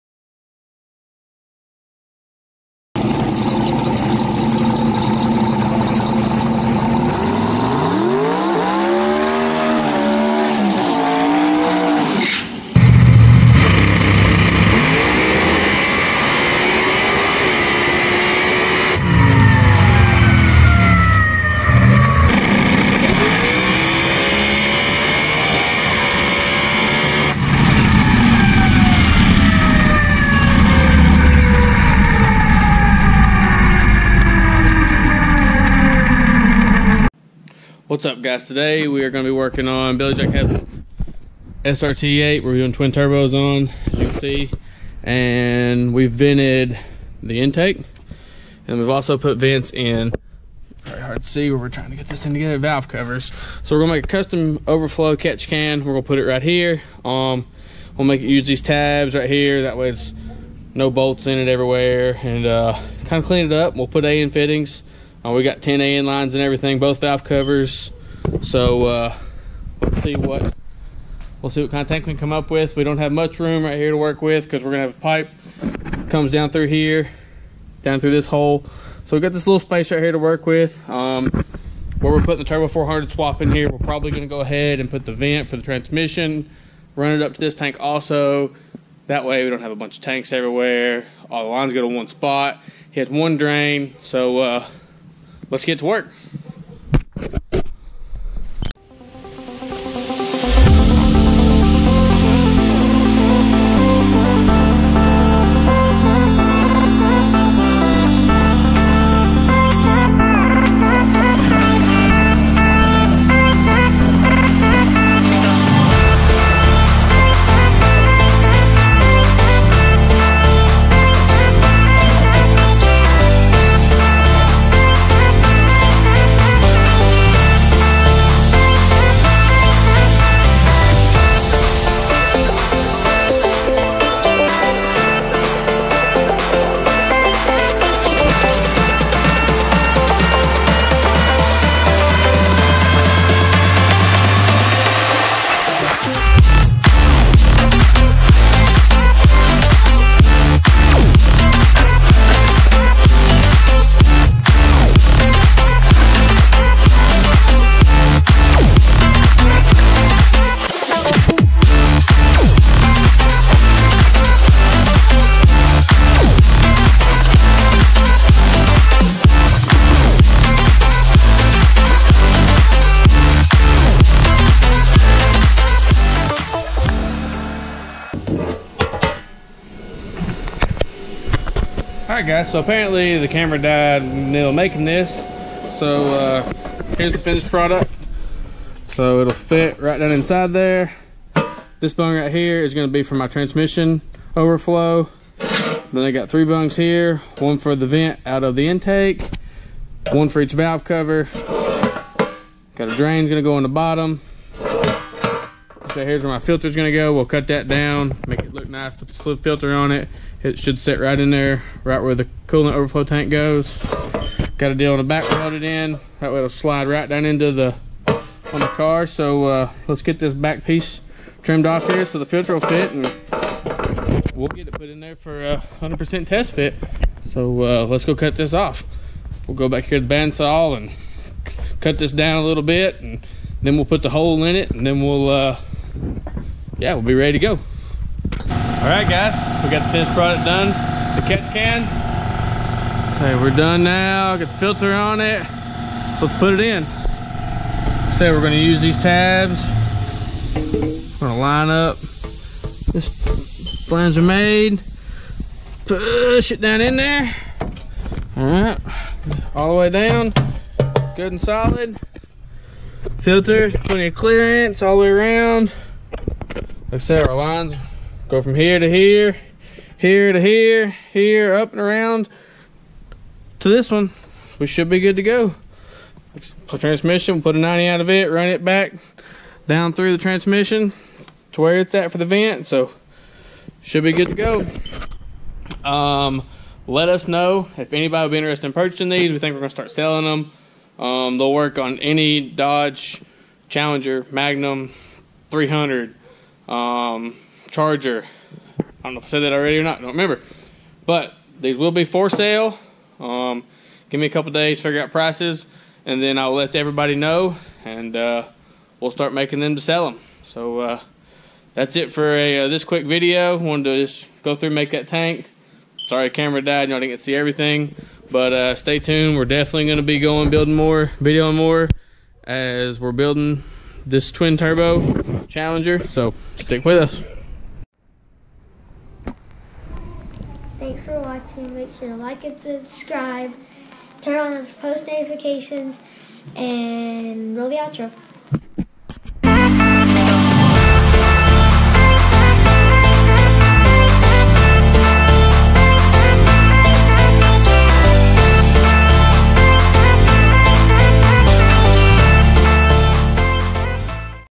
Twin Turbo Dodge Challenger Custom Over Flow Tank
Custom OverFlow Tank We Built For a Dodge Challenger We are building a Twin Turbo Kit on and a Custom TH400 Install